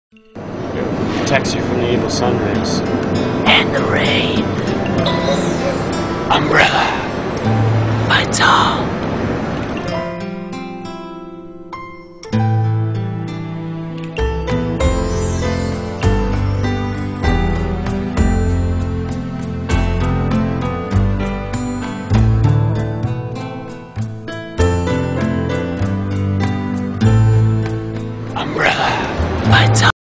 Umbrella by Tom
Commercial for Umbrella by Tom, by Tom Zahara and Tom Smith. Exclusively featuring Michael Zych.